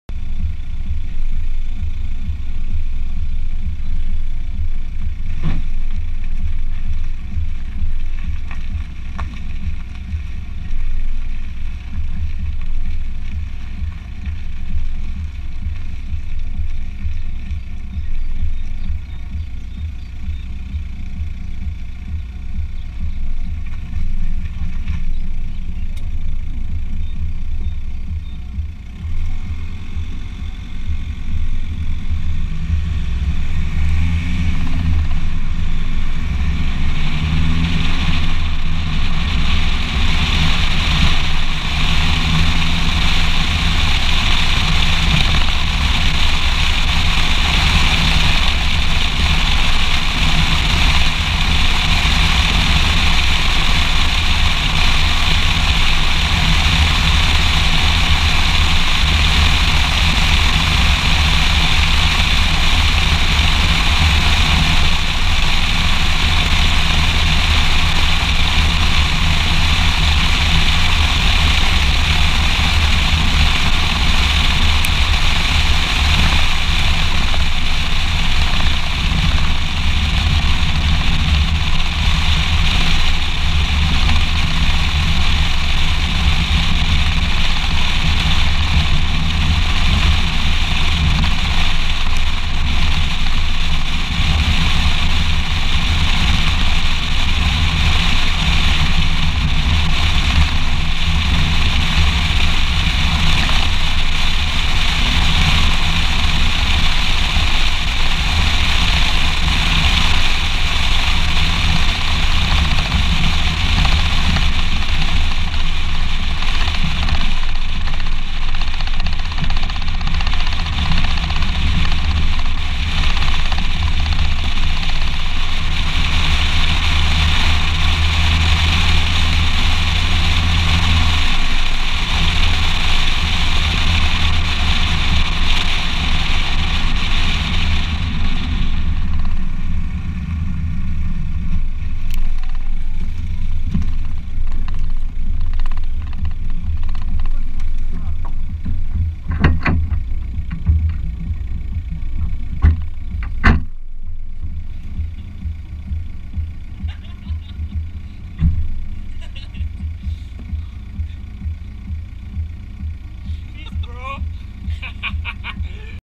GOPR0 Audi a4 b6
W filmiku testowana jest kamerka GoPro 4 silver